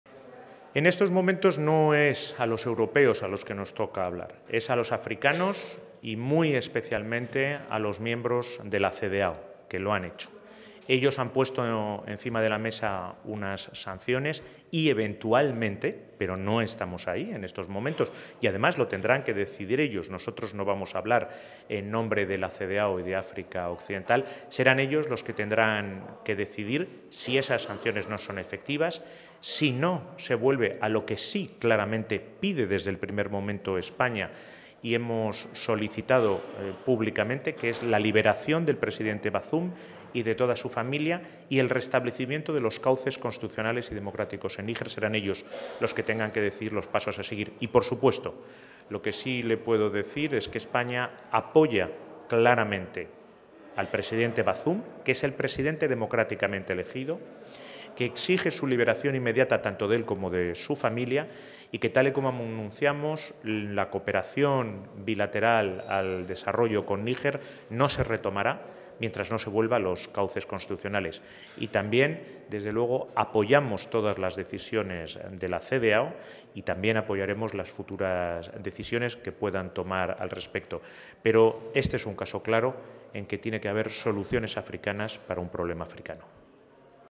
España apoya las sanciones africanas por el golpe de estado en Níger
El ministro de Asuntos Exteriores, Unión Europea y Cooperación en funciones, José Manuel Albares, ha trasladado este lunes el apoyo de España a las sanciones que la Comunidad Económica de Estados de África Occidental (CEDEAO) ha impuesto contra individuos y organizaciones vinculados a la junta golpista de Níger.

(Fuente: Congreso)